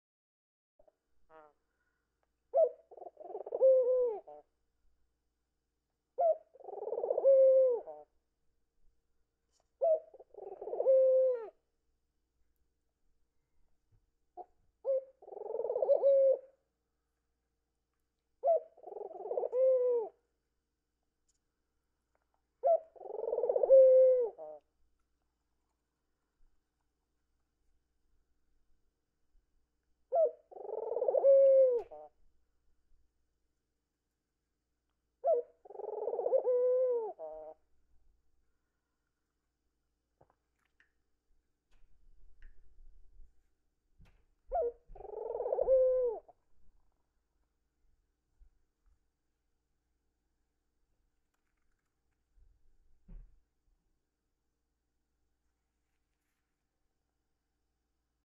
Tango Wing Twitching & Cooing
Tango wanting our attention. LOL.